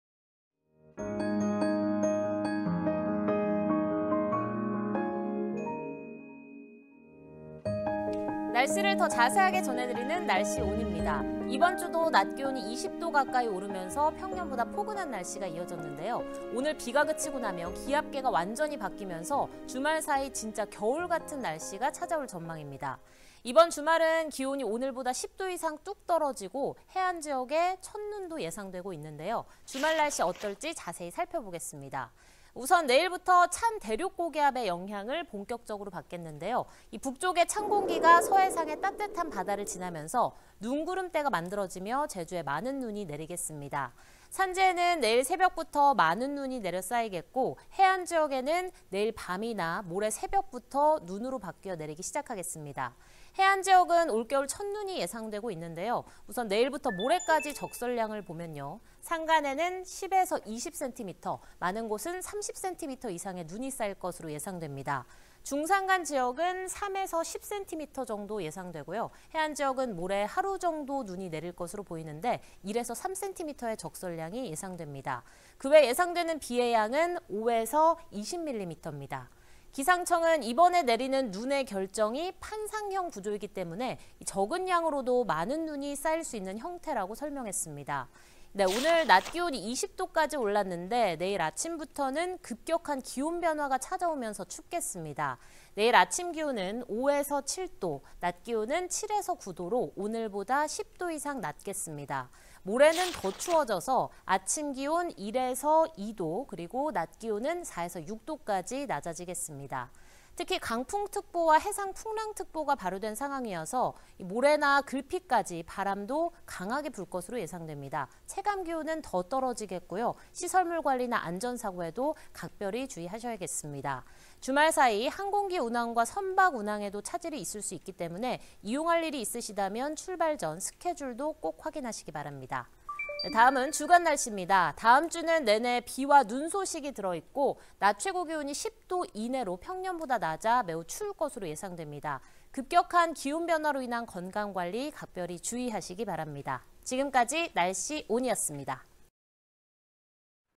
날씨ON(본격적인 겨울 추위, 해안 첫눈 예상)
날씨를 더 자세하게 전해드리는 날씨온입니다.

이번주도 낮기온이 20도 가까이 오르면서 평년보다 포근한 날씨가 이어졌는데요.

오늘 비가 그치고 나면 기압계가 완전히 바뀌면서 주말사이 진짜 겨울같은 날씨가 찾아올 전망입니다.